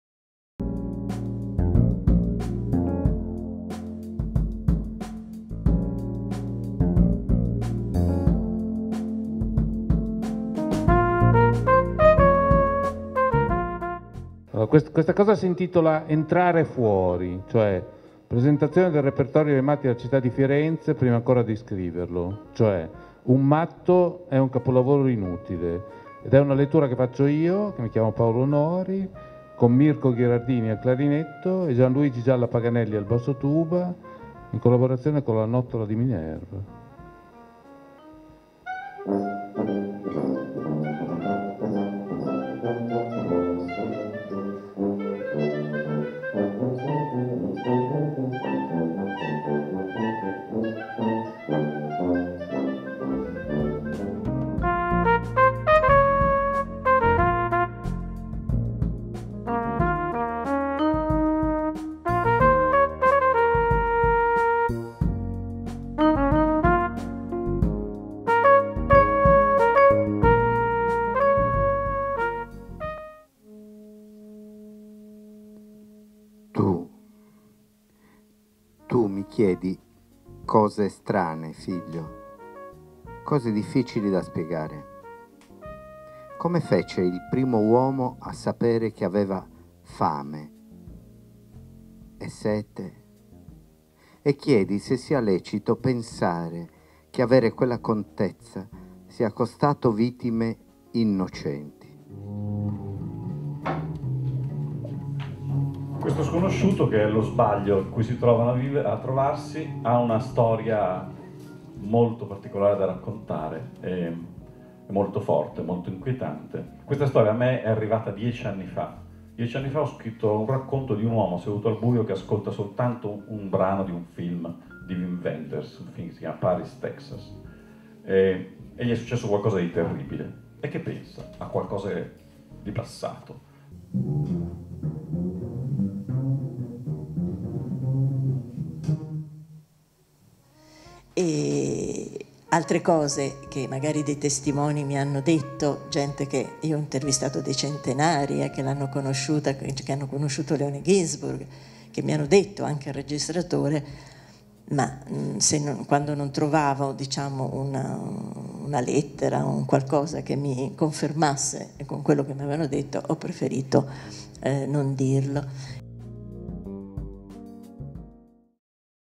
Reading in the Sound 2018
Follia, poesia, amore, storia e impegno culturale e politico: cinque incontri per scoprire il quotidiano nella straordinaria forza motrice della letteratura.